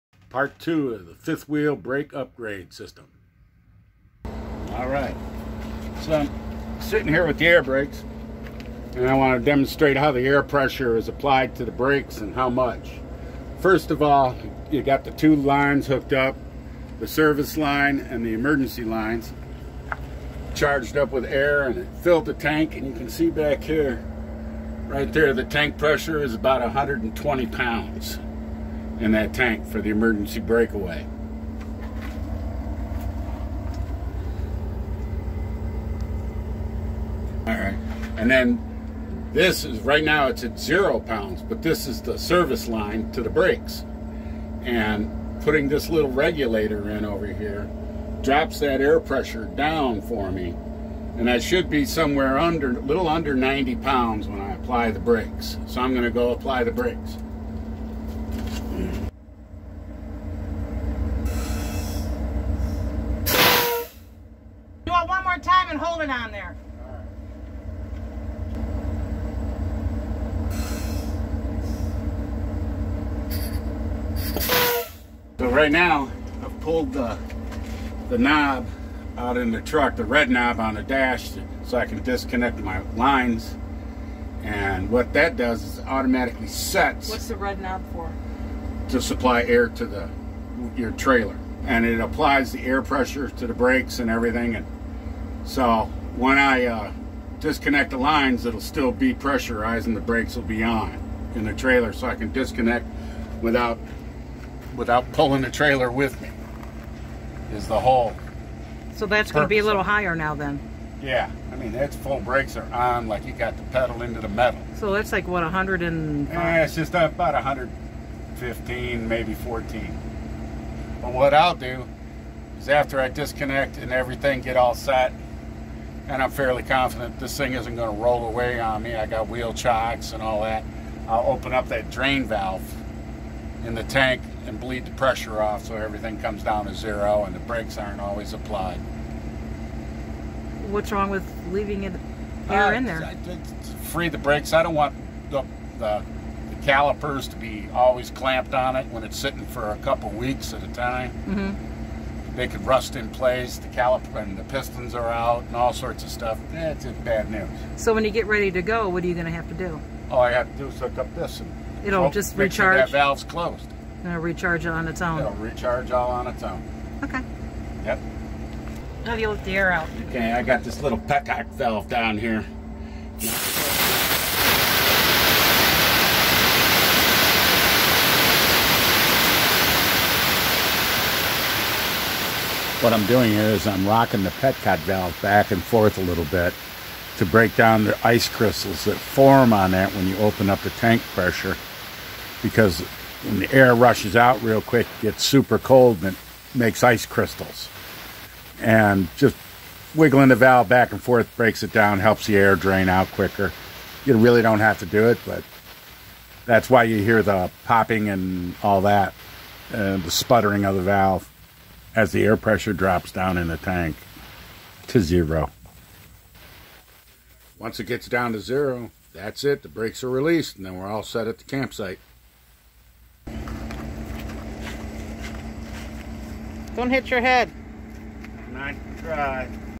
Part 2 - BluDot RV Air Brake System for RV (Pressure Check)
This Part 2 short video shows how to check the pressure of the BluDot Air Brake System for RVs once installed.  Part 1 is the video linked below which is how the BluDot Brake System is installed.